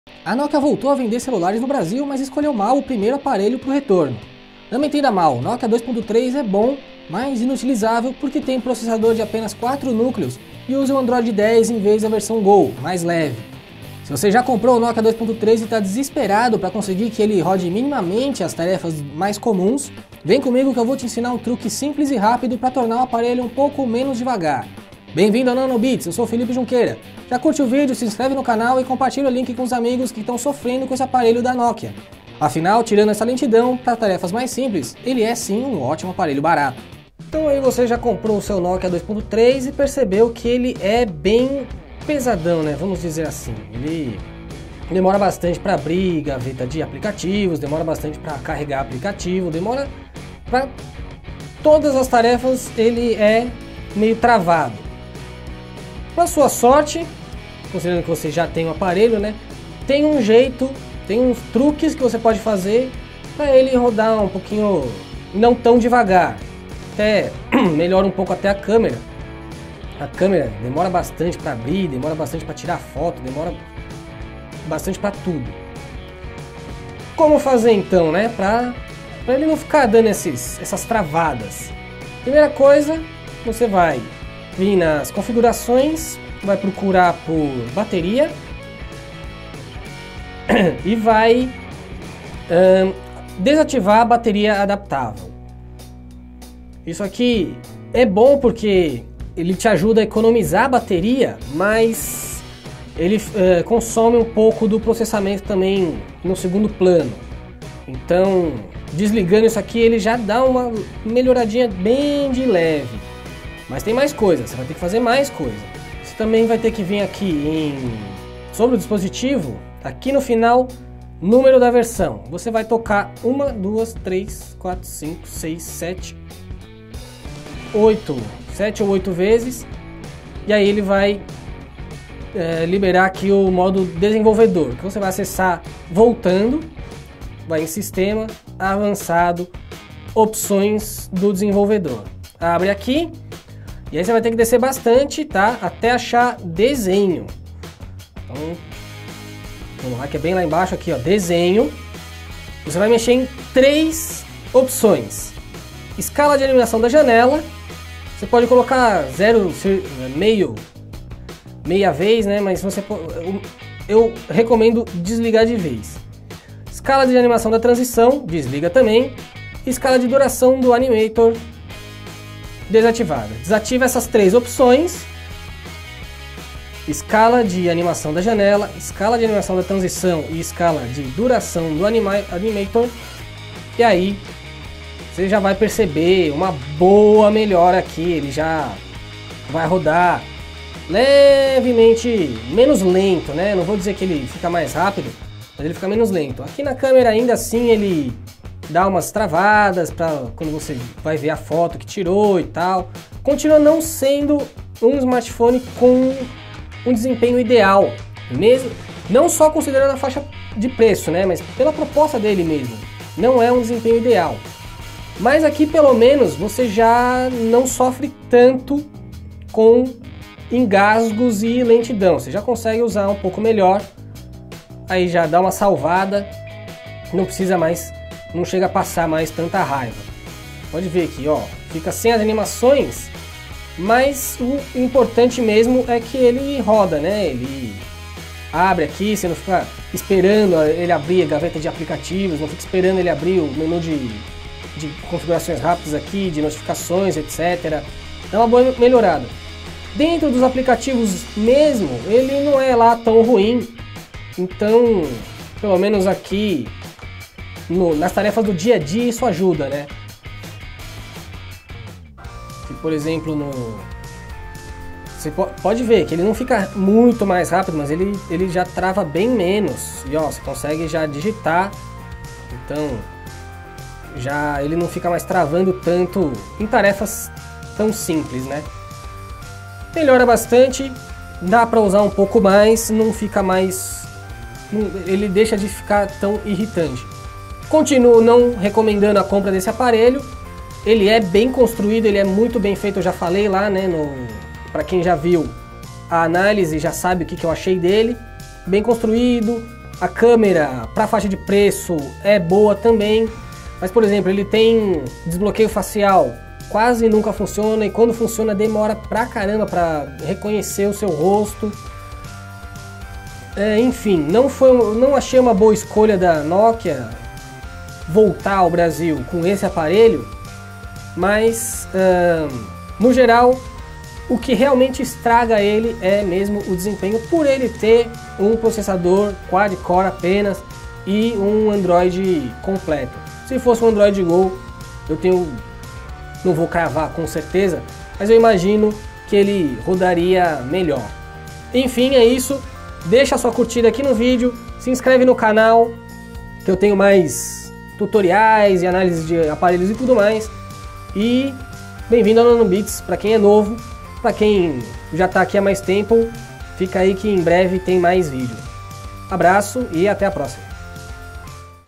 Como deixar o Nokia 2.3 mais rápido (ou travando menos)
A Nokia não escolheu muito bem o primeiro celular para voltar a vender no Brasil. Mas não faz mal: tem um truque para o seu Nokia 2.3 rodar um pouco melhor e travar menos nas tarefas mais simples, até se tornando utilizável! Segue o vídeo, é bem simples e rápido :)

Compre pela internet e ajude o nanobits!